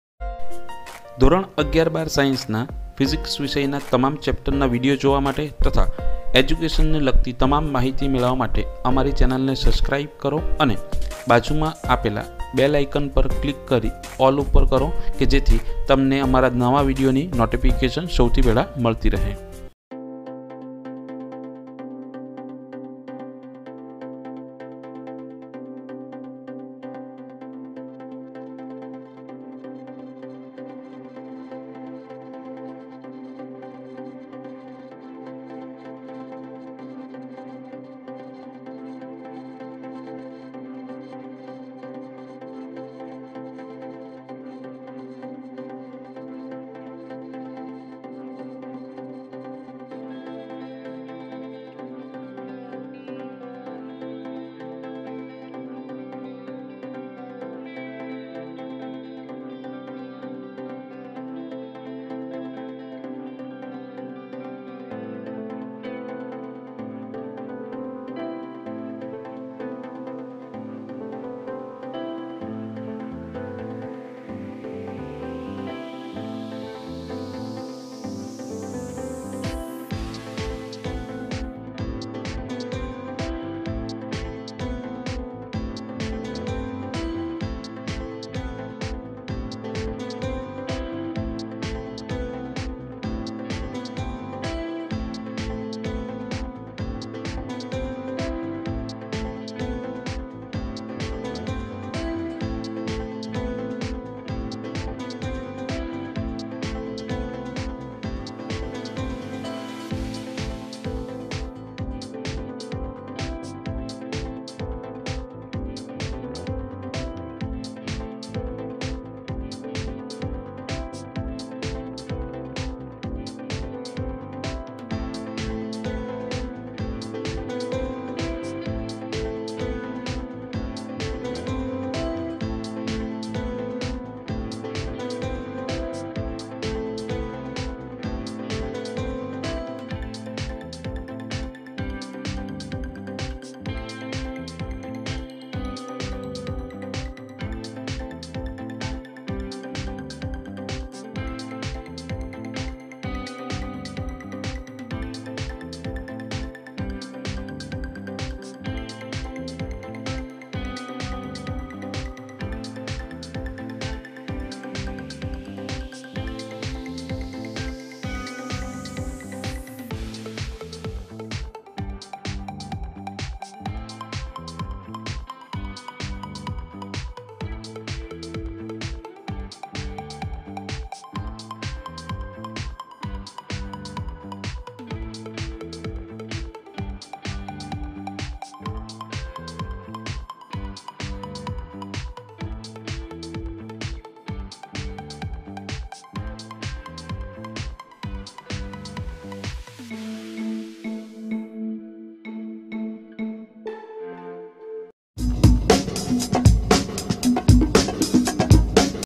Mathematics Paper Solution | Standard-12 Science Maths Paper Solution | October-2023 SVS Exam Paper
Correction: 46-C

◆Video Publish Time : 2 pm (11/10/23)

HI,
I am Prakash R Purohit..

Topic  : 12th Science SVS Maths Paper Solution

This Video is about Standard:12th Science Gujarat board (GSHSEB) NCERT syllabus.(Gujarati Medium )

It Will Helps you for  preparation of GUJCET , NEET , JEE and Board Exams.

Telegram : user name : @prakashrpurohit